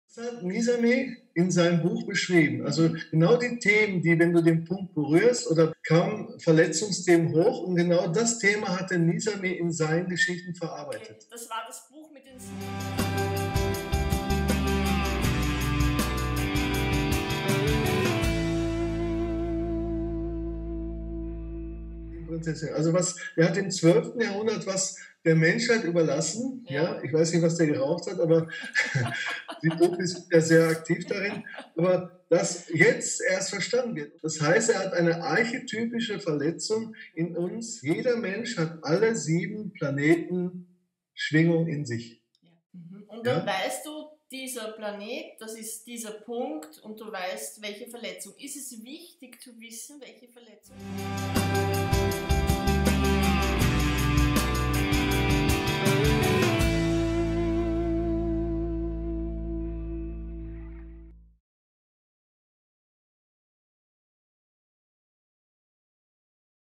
Interview Häppchen: Die Liebe als ...
... Heilprozess. Verletzungen der Liebe! Ein Einblick ins Interview mit Stephan Hafiz Kugel. Noch mehr Info zu "Beziehung braucht Stimmung"